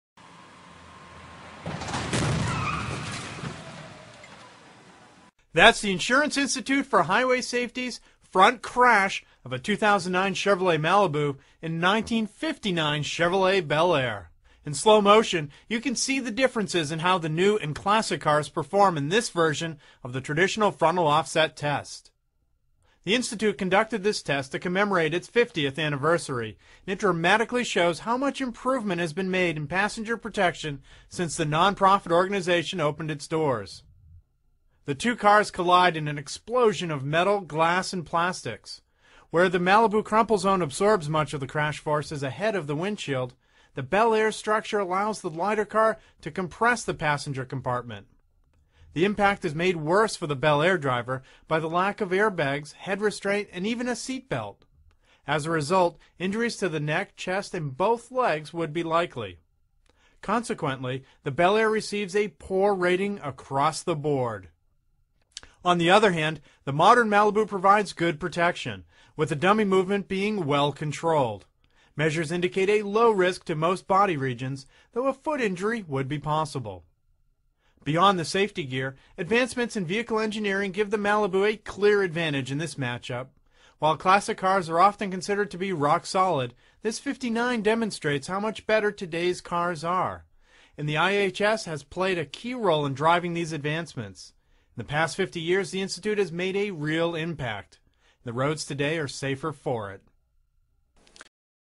1959 Chevrolet Bel Air vs 2009 Chevrolet Malibu Crash Test
In the 50 years since US insurers organized the Insurance Institute for Highway Safety, car crash-worthiness has improved. Demonstrating this was a crash test conducted on Sept. 9 between a 1959 Chevrolet Bel Air and a 2009 Chevrolet Malibu. In a real-world collision similar to this test, occupants of the new model would fare much better than in the vintage Chevy.

The crash test was conducted at an event to celebrate the contributions of auto insurers to highway safety progress over 50 years. Beginning with the Institute's 1959 founding, insurers have maintained the resolve, articulated in the 1950s, to "conduct, sponsor, and encourage programs designed to aid in the conservation and preservation of life and property from the hazards of highway accidents."